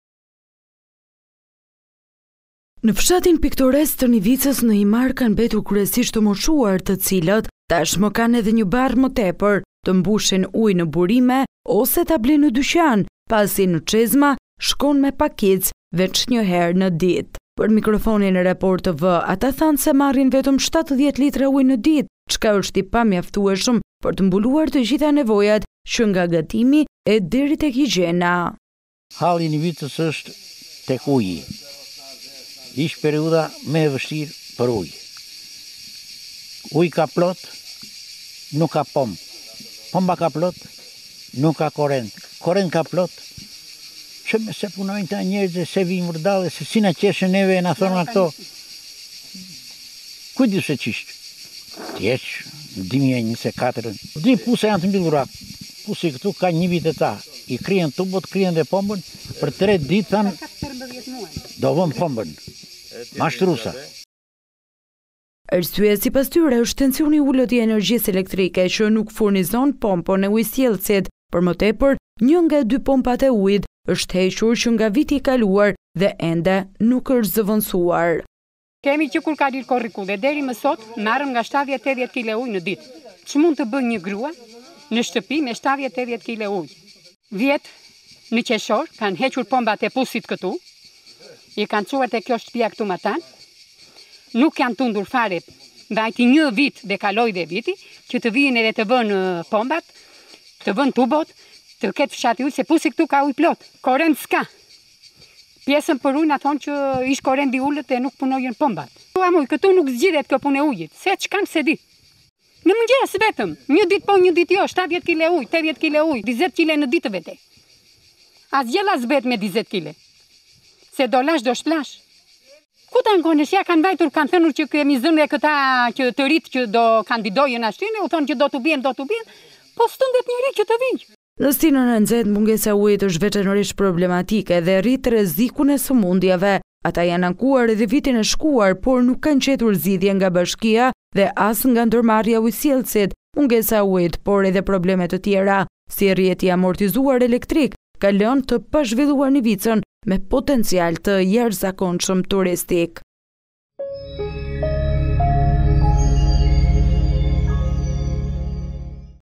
Banorët e Nivicës në Himarë: Çfarë mund të bëjë një familje me 70 litra ujë në ditë?!
Kur temperaturat shënojnë 40 gradë, banorët e Nivicës në Himarë po përballen me thatësirën. Uji në çezma shkon vetëm një herë të ditë, ndërsa banorët kryesisht të moshuar detyrohen të mbushin në çezma publike apo ta blejnë ujin e pijshëm.